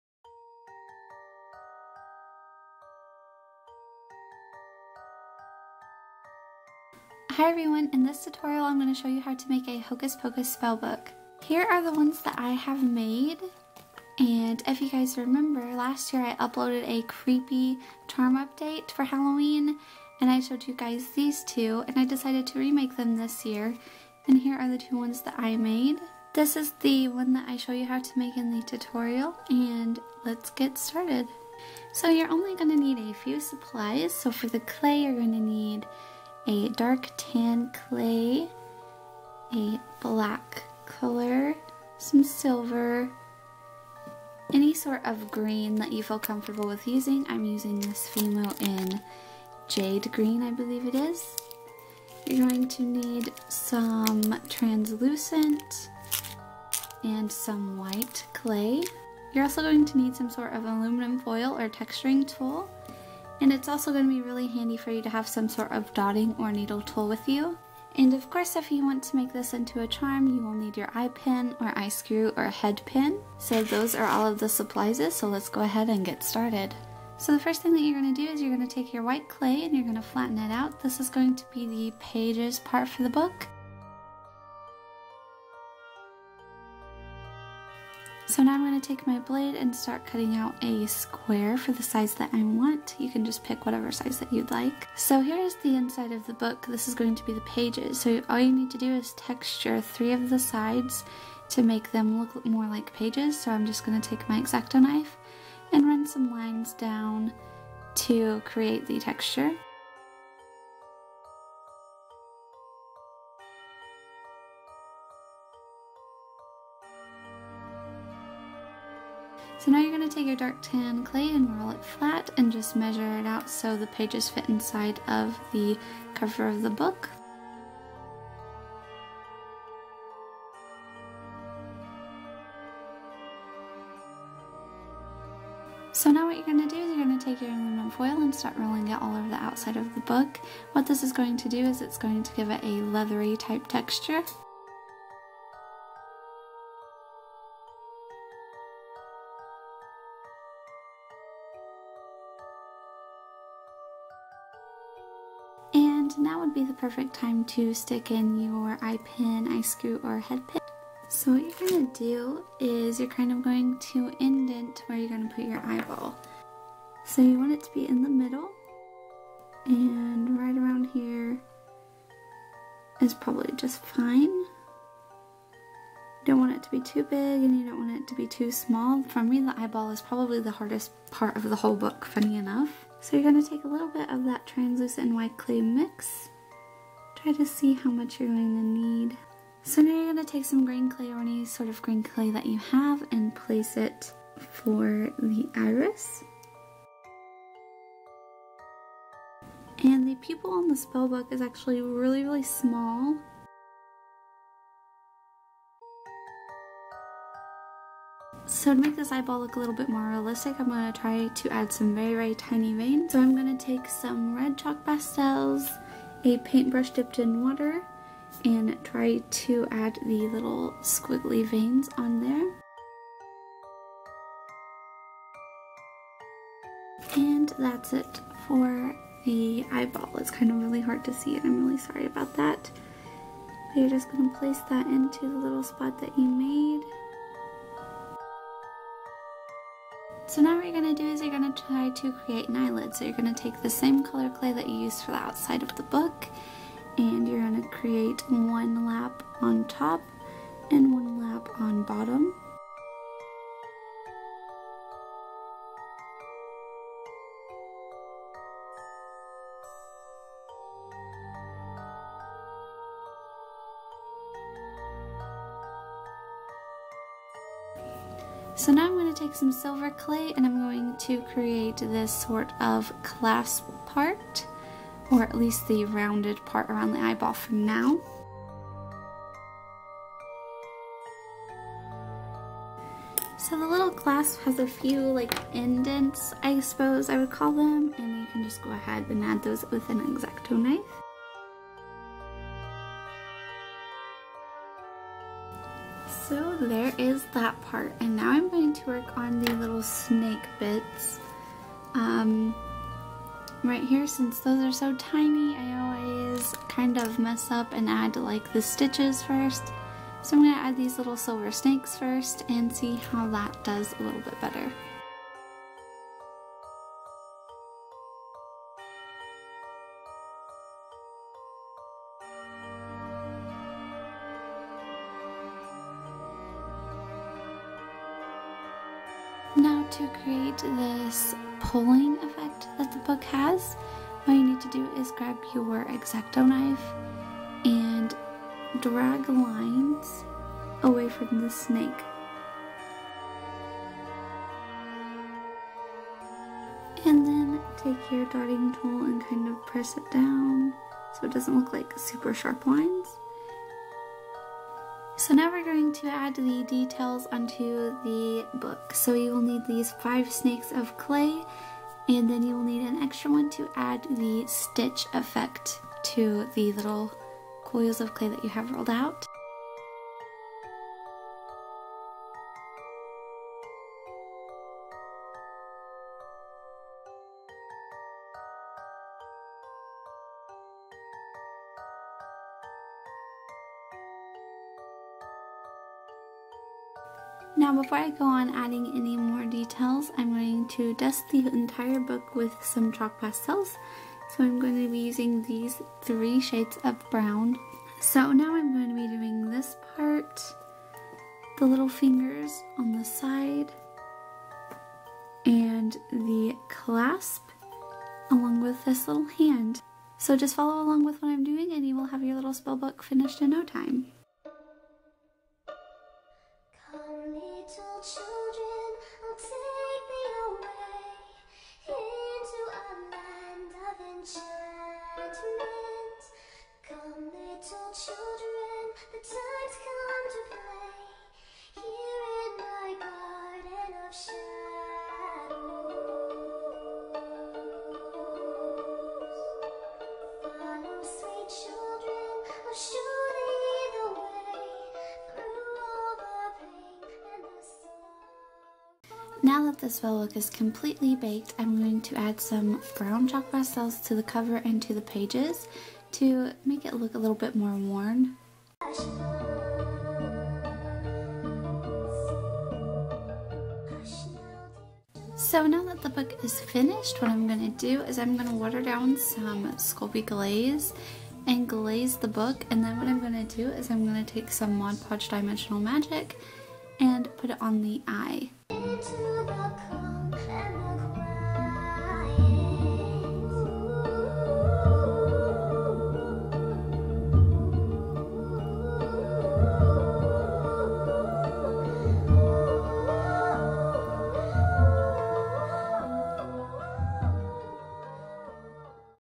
How To Make a Hocus Pocus Spell Book out of Polymer Clay [Tutorial]
Hi everyone! In todays video I show you how to make a Hocus Pocus spell book! If you've been following my channel you probably know that I love making spell books! This video is kind of old (I found the lost footage on my old laptop) but the info is still correct on how I make these charms. I hope you all enjoy~!
If you make your own charm inspired by me tag me on Instagram @cosmicwishes I'd love to see your creations!

HEY You! Quick question about a future video: Do you like ASMR (whispering/sounds/white noise) videos? If so let me know!  (that is if anyone is reading this :p )  I have one in the works but I'm not sure if I should post it~

☆☆ Social Media ☆☆

♥ Crafty Amino: CosmicWishes

ღ S U B S C R I B E ღ